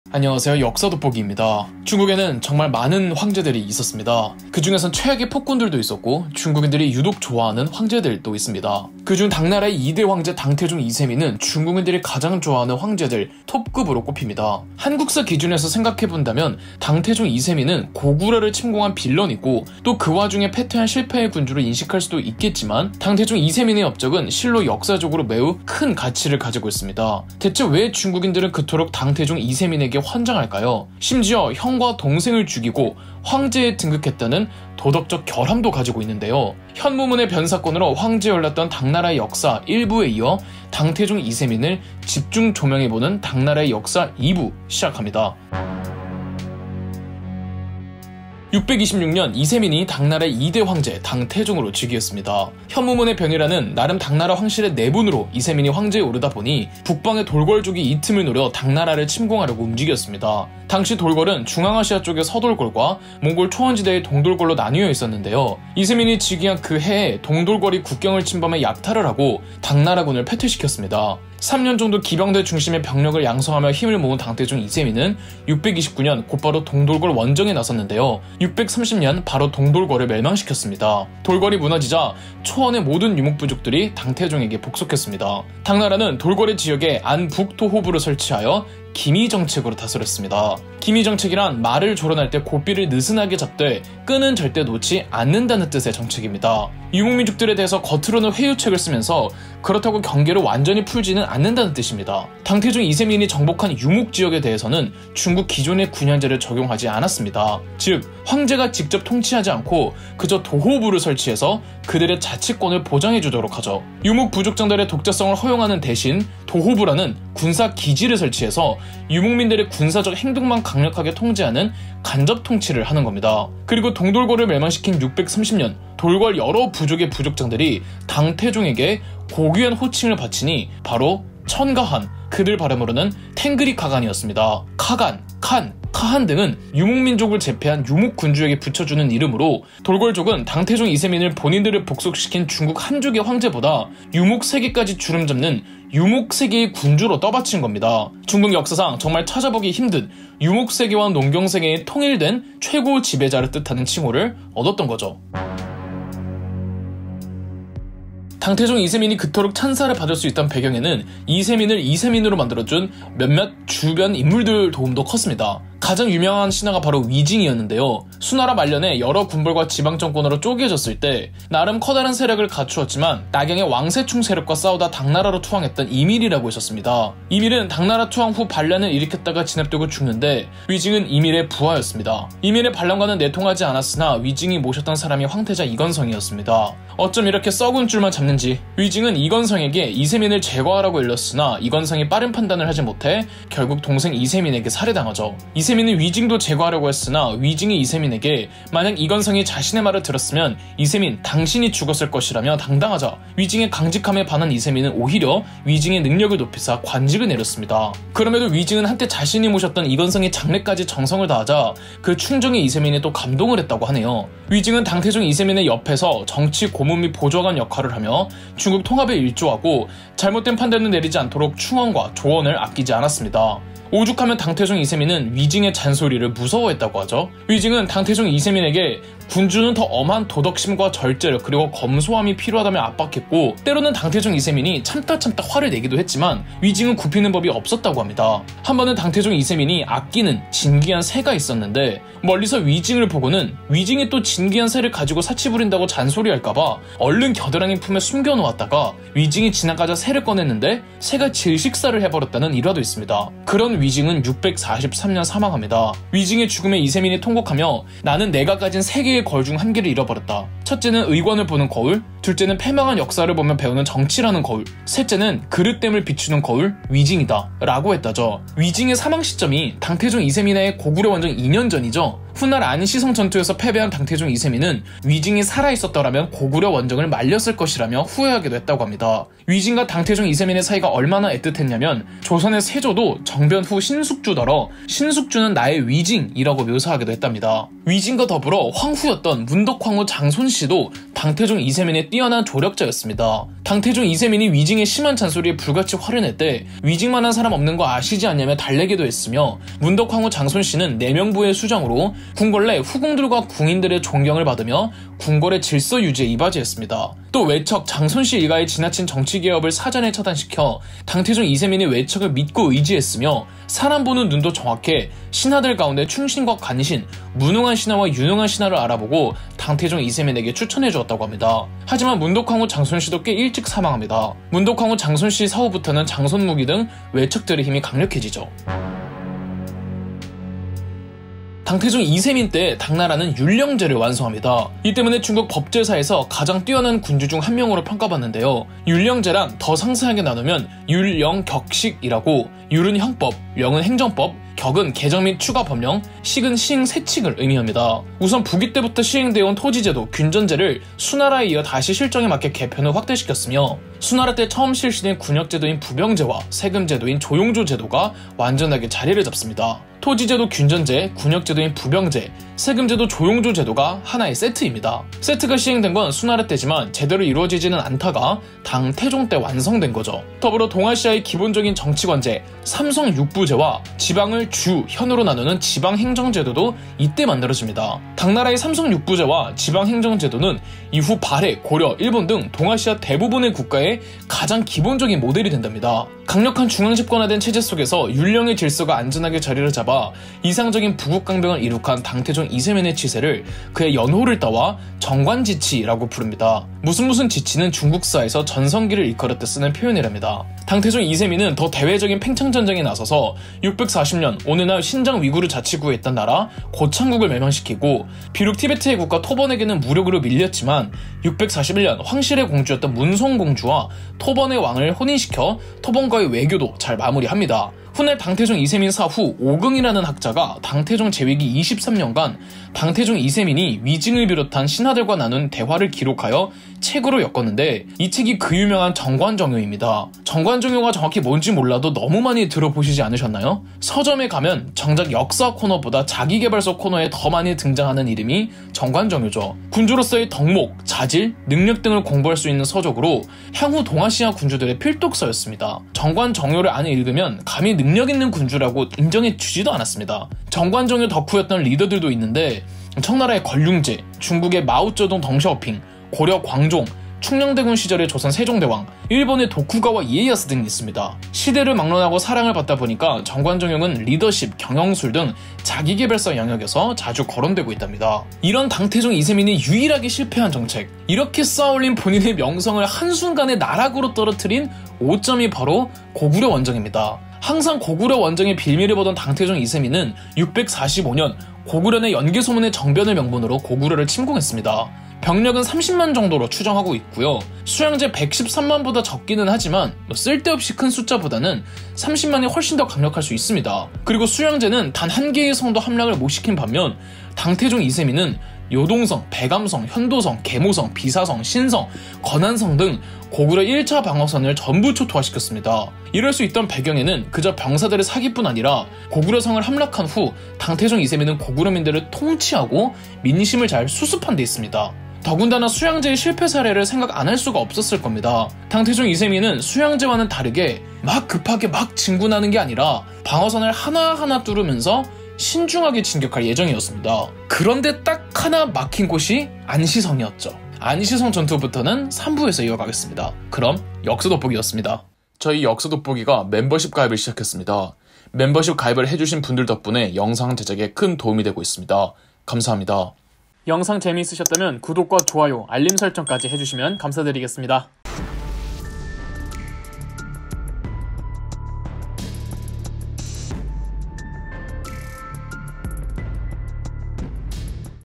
동아시아 군주들의 롤 모델 당 태종 이세민의 23년간의 재위기 당나라의 역사 2부
당나라의 역사 2부.
중국인들은 왜 당 태종 이세민에게 환장할까

그럼 역사돋보기 시작합니다.

도서 리뷰 및 기타 협업 제의 있으신 분 연락 주세요.
감사합니다 :)